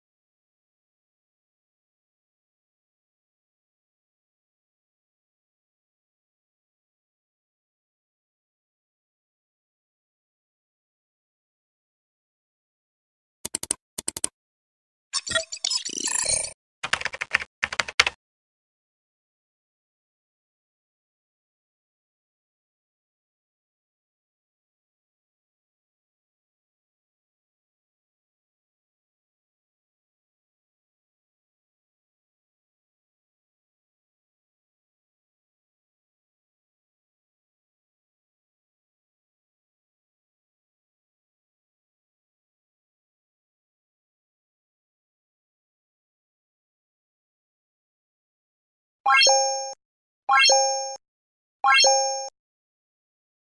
Choose 4 movies wisely for live stream on BIGO live.
If you don't know where it is. Go to my channel, find community and you should find the vote of Winnie the Pooh, Sonic 1 or 2, Disenchanted and Dragons 3.
Also, there some information about it and remember choose wisely and see you in BIGO live.